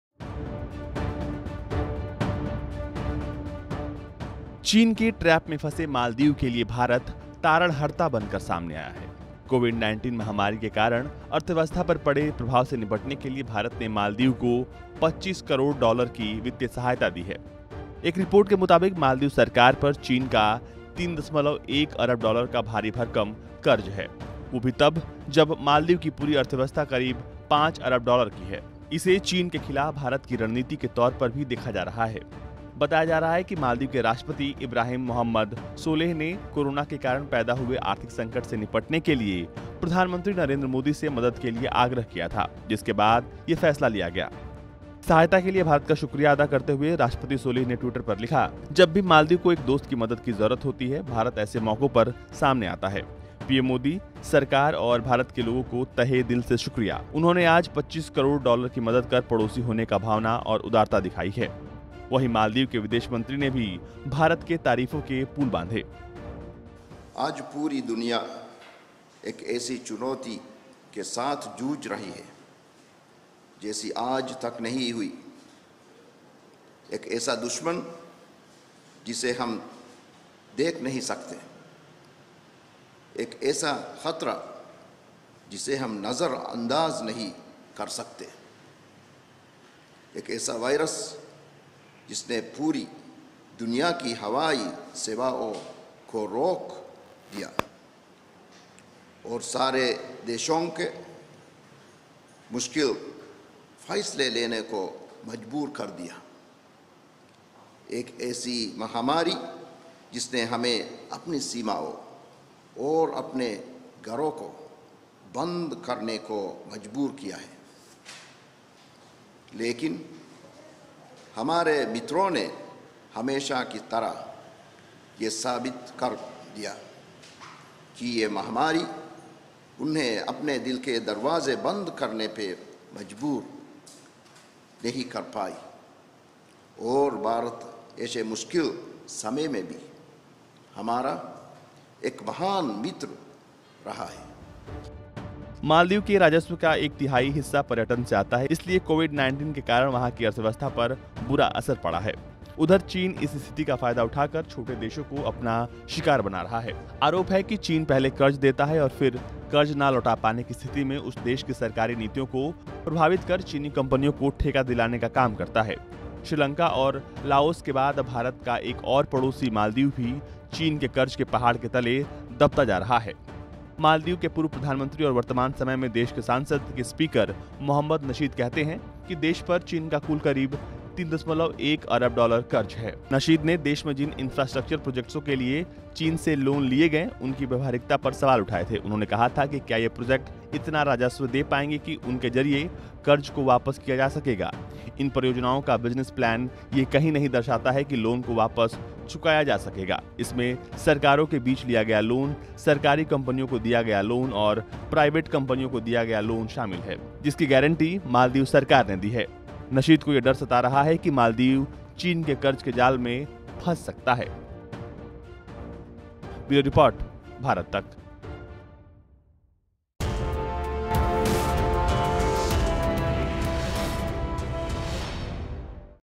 चीनी कर्ज में फंसे मालदीव को भारत ने दी 25 करोड़ डॉलर की मदद
कोविड -19 महामारी के कारण अर्थव्यवस्था पर पड़े प्रभाव से निपटने के लिए भारत ने मालदीव को 25 करोड़ डॉलर की वित्तीय सहायता दी है.